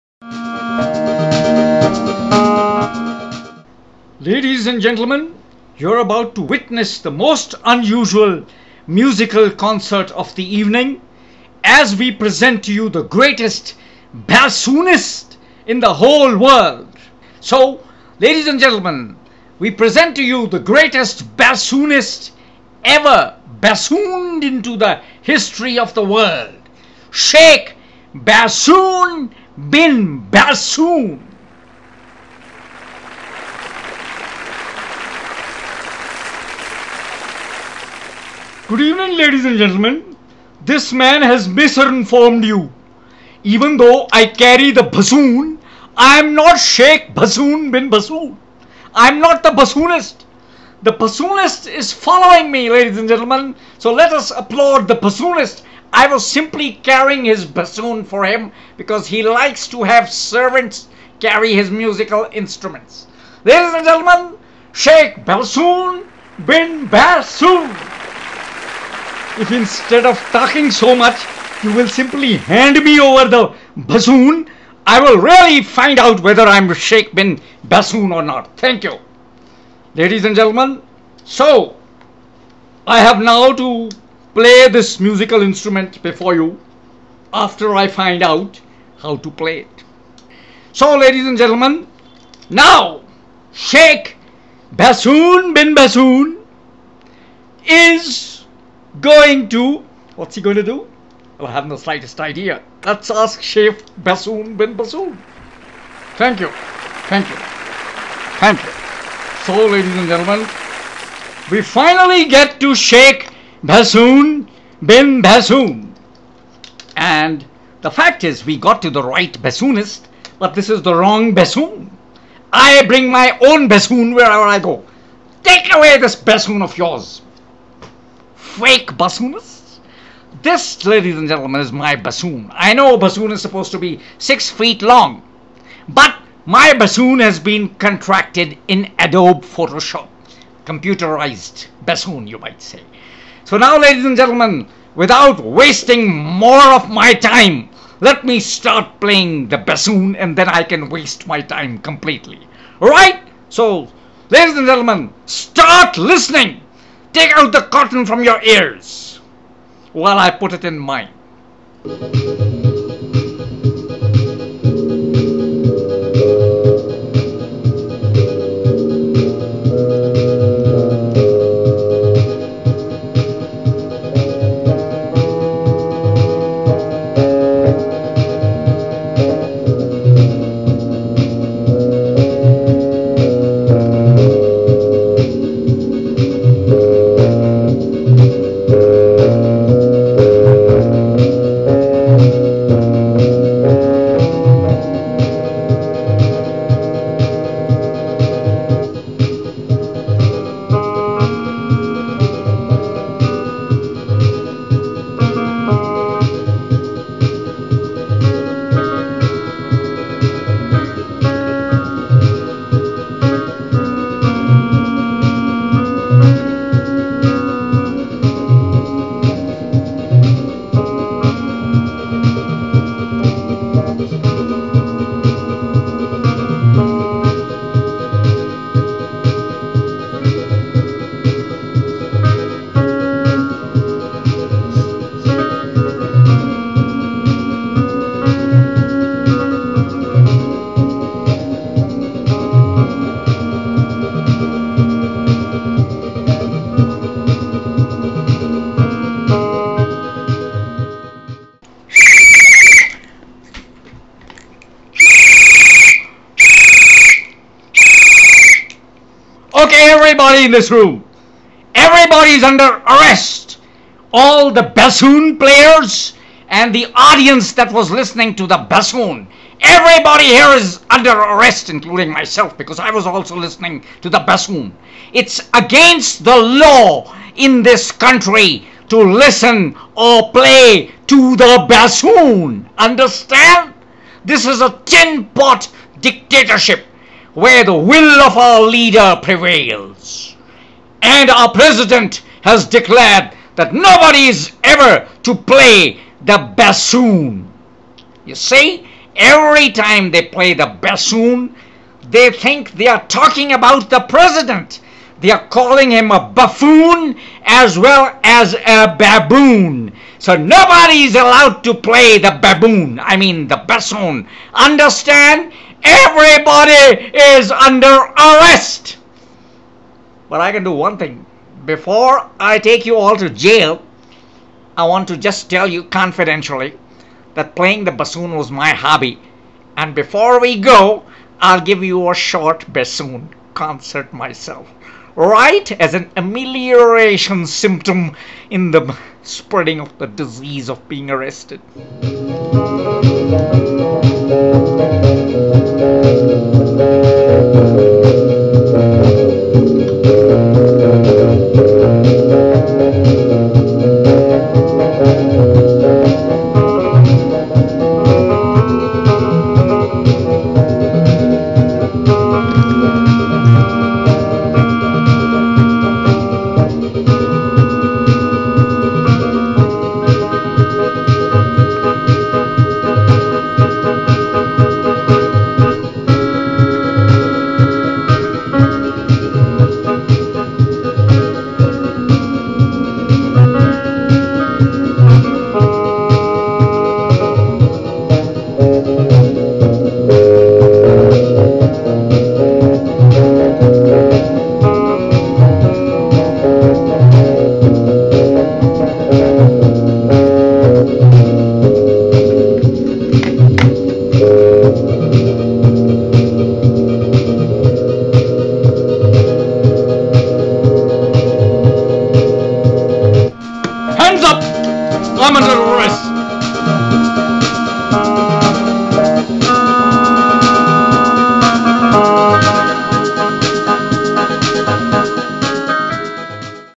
SPOOF/PARODY - BASSOONIST
Here is the greatest bassoonist in the world - Sheikh Bassoon Bin Bassoon --and the weird aftermath to his concert!